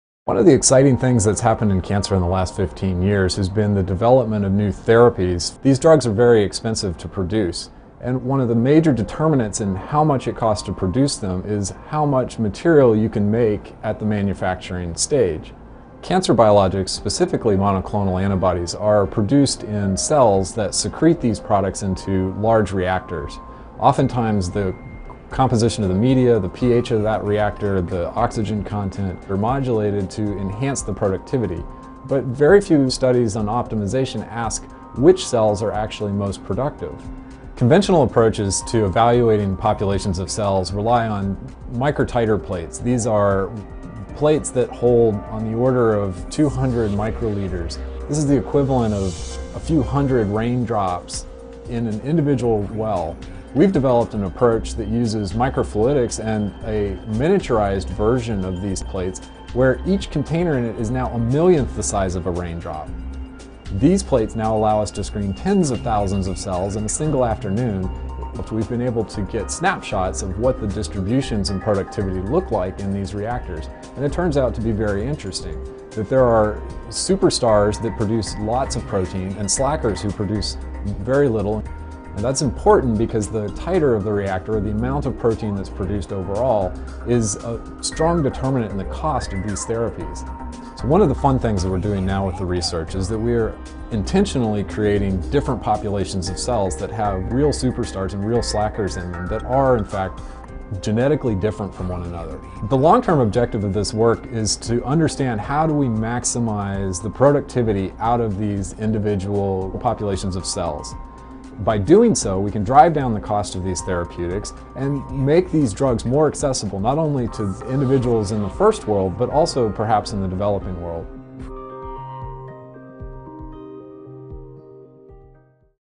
Inside the Lab: J. Christopher Love, Ph.D.
Learn more about how the Love lab is using miniature micro titer plates and micro fluidics to evaluate populations of cells, maximize their productivity, and ultimately improve the process for developing cancer biologics.